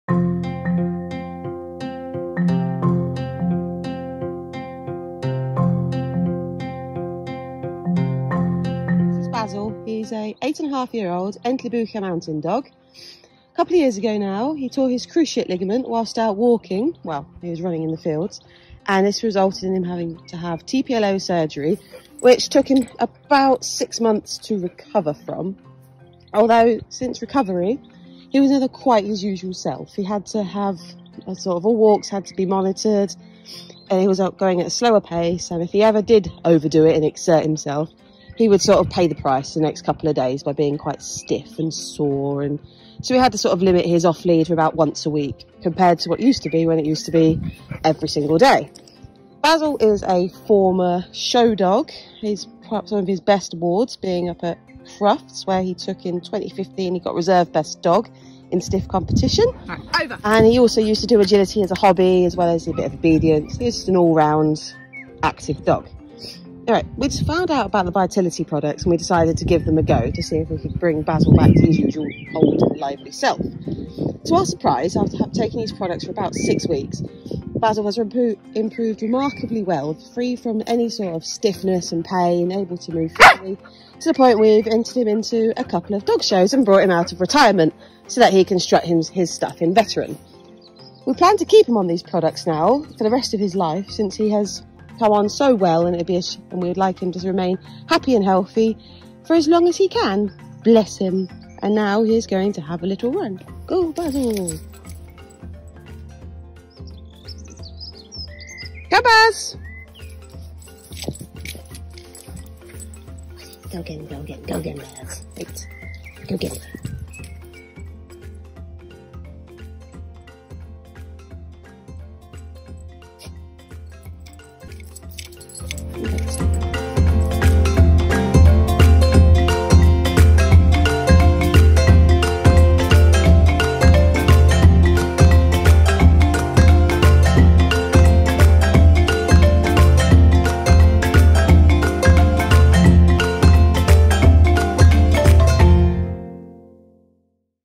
8.5 years old Entlebucher Mountain Dog's recovery from cruciate ligament injury and TPLO surgery
Basil is an 8.5 years old Entlebucher Mountain Dog. He is a former show dog and was Reserve Best Dog in 2015. A couple of years ago he tore his cruciate ligament and had to have TPLO surgery which took him 6 months to recover from. Even after the recovery he couldn't enjoy the daily walks and when he overdid it on playing he paid the price the next day, he was sore and stiff the next morning. When we heard about the Vitality Challenge Trial we decided to give it a try and we're glad we did because Basil is now back to his old self, energetic and loves his runs. Thanks to the team at Dogs Today Magazine for the opportunity and BiogenicPet Vitality for developing a great product that really works!